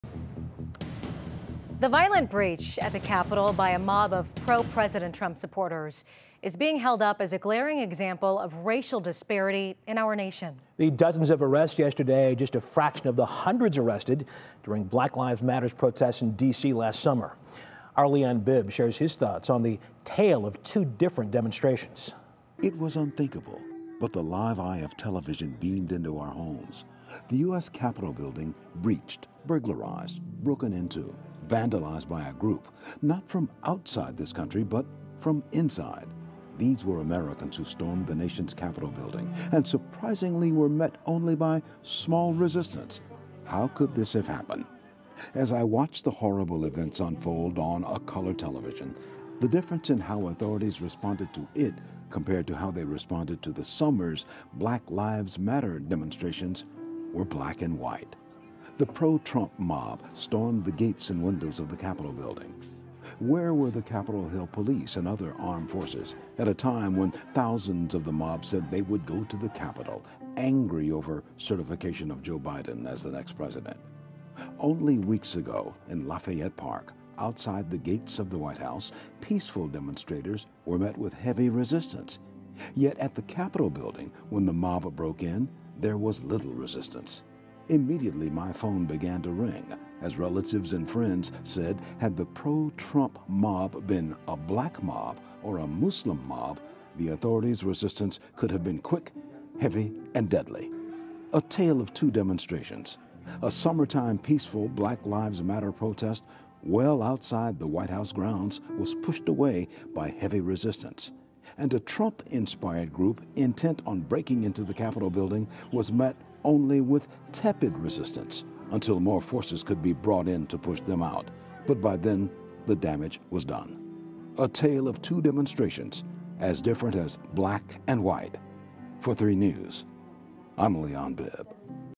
Racial disparity and the tale of two different demonstrations
The dozens of arrests yesterday are just a fraction of the hundred arrested during BLM protests in DC last summer. Leon Bibb shares his thoughts